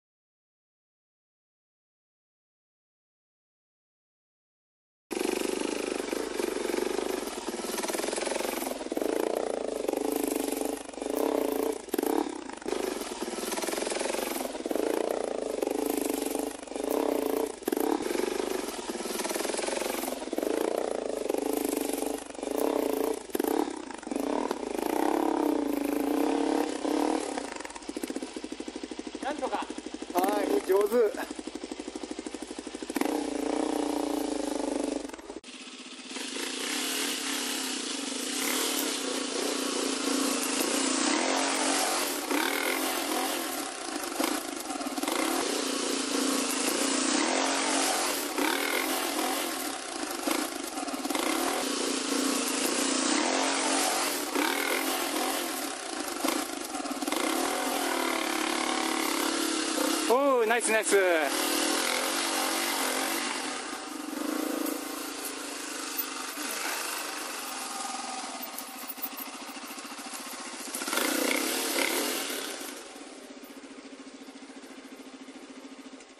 倒木をエスケープ
目の前に倒木が・・・引き返していたら約束の帰宅時間に間に合わない・・・仕方が無く木々の間をエスケープ。 もちろん、轍はきれいに直しておきました。